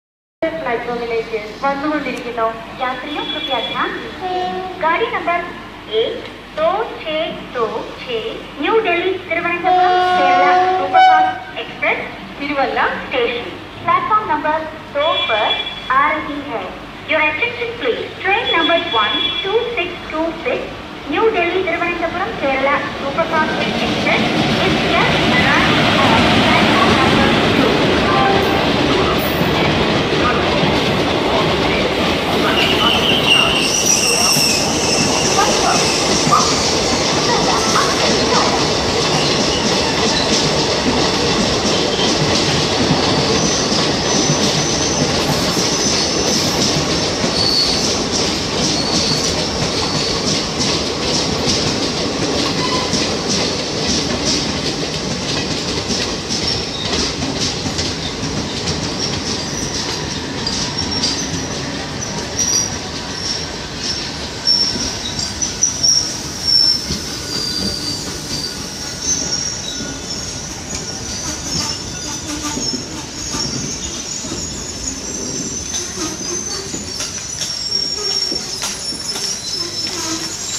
Kerala Express mass entry with announcement @ Tiruvalla
Location: Tiruvalla railway station, Pathanamthitta, Kerala.
Date & Time: 9th July 2016 02:51PM

Erode (ED) WAP4 22294 hauled 12626 New Delhi - Thiruvananthapuram Central Kerala Superfast Express arriving Thiruvalla station (train entry announcement)